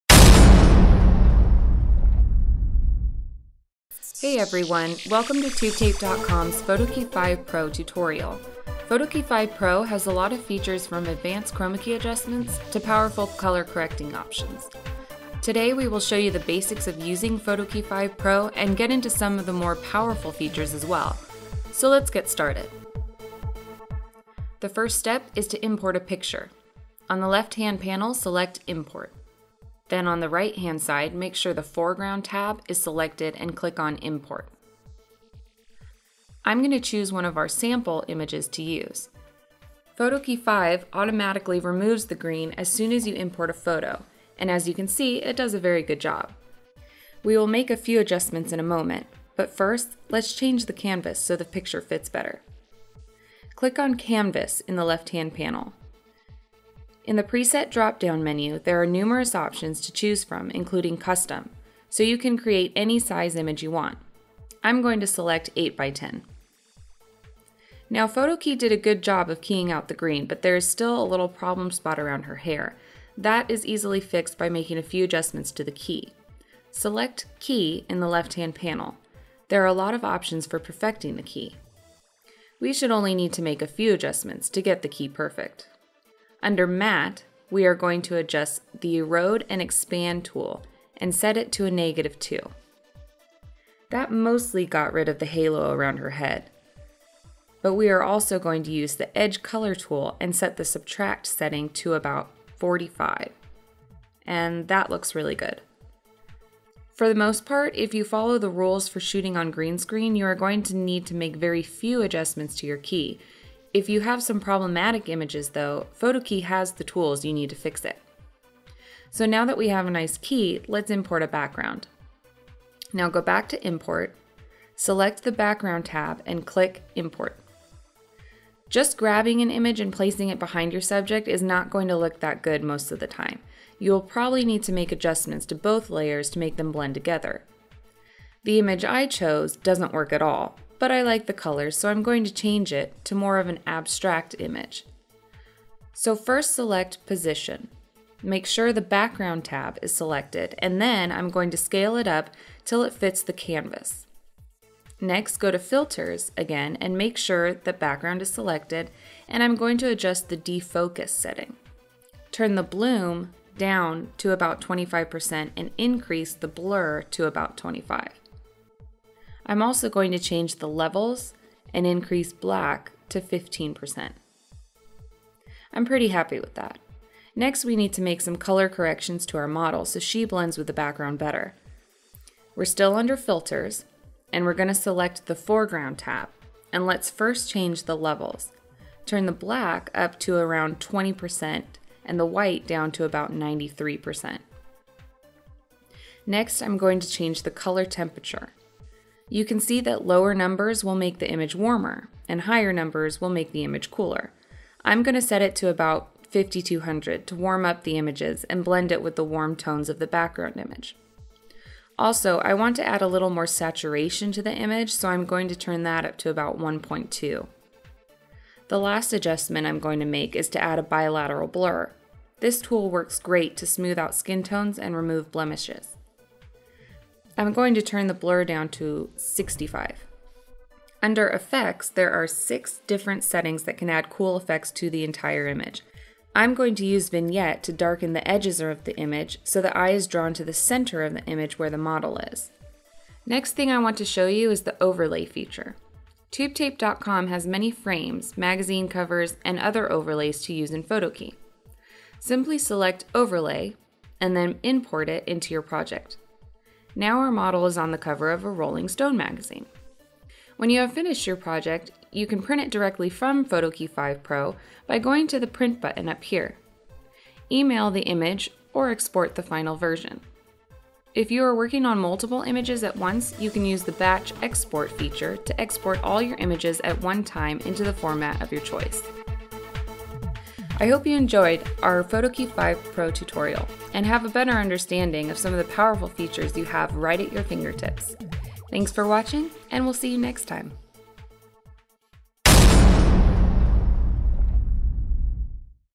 PhotoKey Pro tutorial
Learn some keying techniques, and color correcting tips in FX Home's PhotoKeyPro.